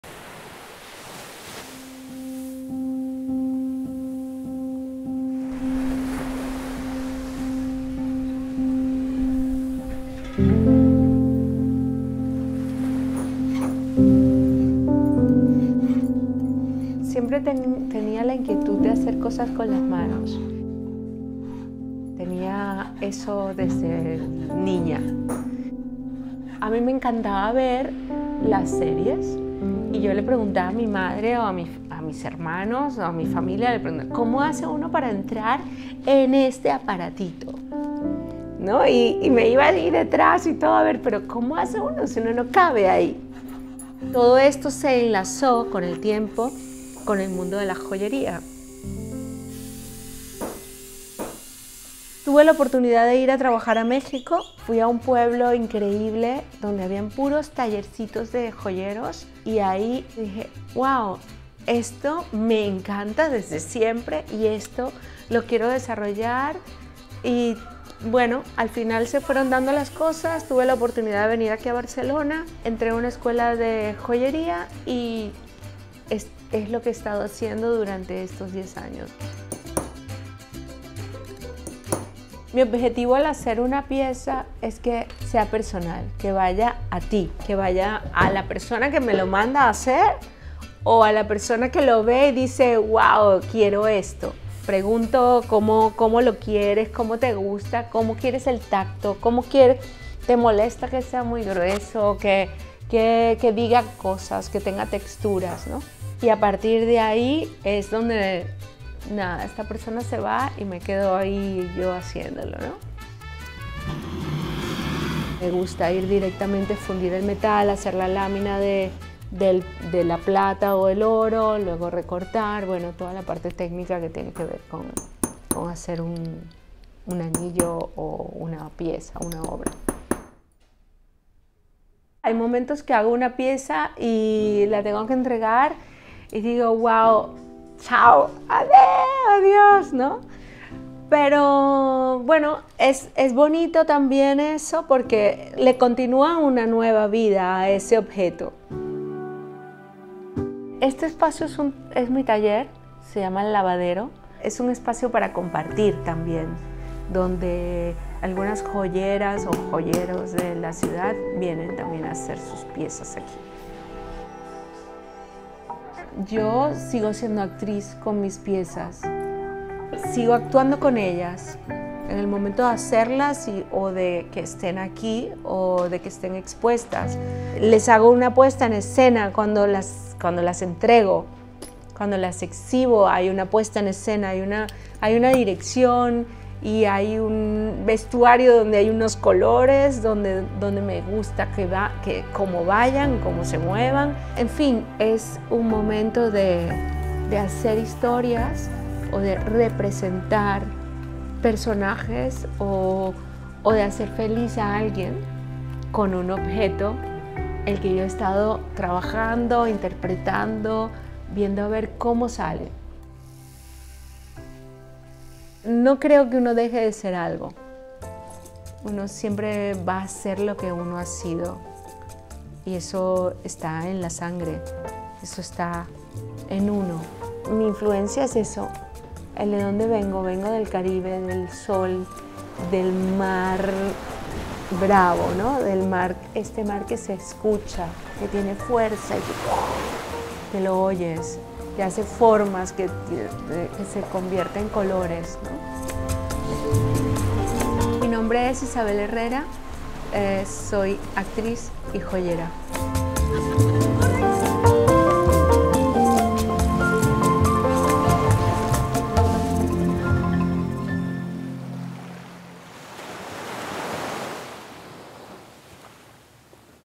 Mi Oficio: Isabel Herrera -Joyera
Isabel Herrera más que una joyera/actriz es una artista. Sus joyas son piezas únicas he irrepetibles. Su arte está lleno de pequeñas historias que van formando un cúmulo de ideas que se transforman en piezas artísticas de joyería. Isabel Herrera es una mujer llena de colores, sonidos y de sabor latinoamericano. Su taller de joyas está ubicado en Barcelona, donde también expone sus piezas al público.
Isabel me permitió estar para la elaboración del anillo de compromiso 💍 que le di a mi esposa.
Gracias Isabel por ese regalo 🎁 💝